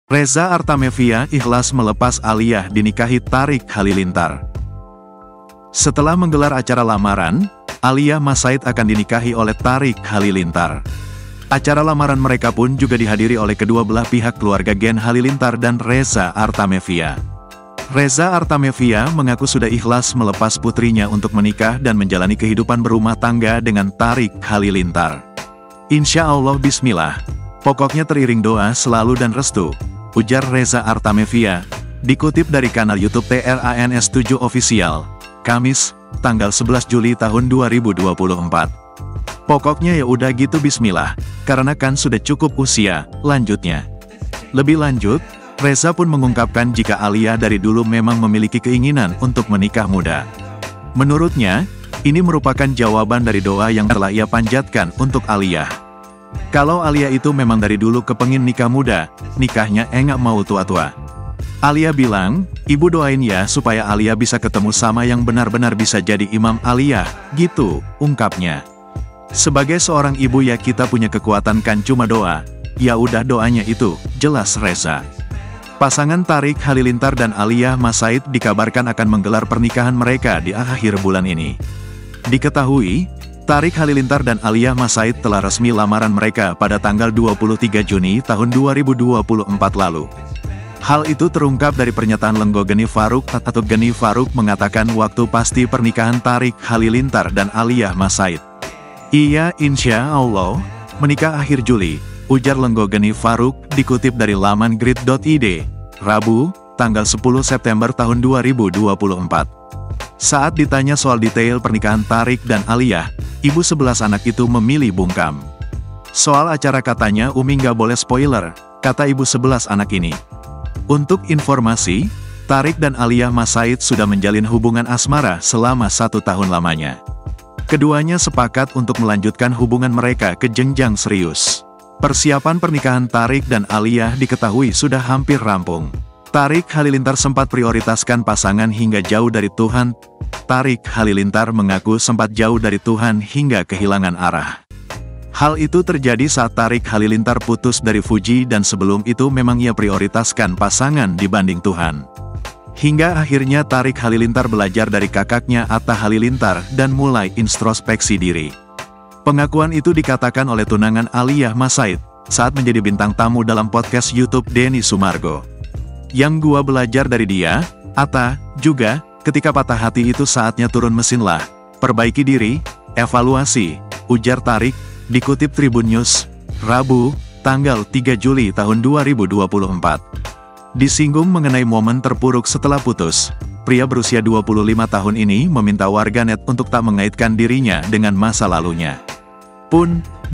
Reza Artamevia Ikhlas Melepas Aaliyah Dinikahi Thariq Halilintar
- Setelah menggelar acara lamaran, Aaliyah Massaid akan dinikahi oleh Thariq Halilintar.
Acara lamaran mereka pun juga dihadiri oleh kedua belah pihak keluarga Gen Halilintar dan Reza Artamevia.
Reza Artamevia mengaku sudah ikhlas melepas putrinya untuk menikah dan menjalani kehidupan berumah tangga dengan Thariq Halilintar.
"Insyaallah bismillah, pokoknya teriring doa selalu dan restu," ujar Reza Artamevia, dikutip dari kanal YouTube TRANS7 OFFICIAL, Kamis (11/7/2024).
"Pokoknya ya udah gitu bismillah, karena kan sudah cukup usia," lanjutnya.
Lebih lanjut, Reza pun mengungkapkan jika Aaliyah dari dulu memang memiliki keinginan untuk menikah muda.
Menurutnya, ini merupakan jawaban dari doa yang telah ia panjatkan untuk Aaliyah.
"Kalau Aaliyah itu memang dari dulu kepengin nikah muda, nikahnya engak mau tua-tua. Aaliyah bilang 'Ibu doain ya supaya Aaliyah bisa ketemu sama yang benar-benar bisa jadi imam Aaliyah' gitu," ungkapnya.
"Sebagai seorang ibu ya kita punya kekuatan kan cuma doa, yaudah doanya itu," jelas Reza.

Editor VIdeo: Rio Angga Saputra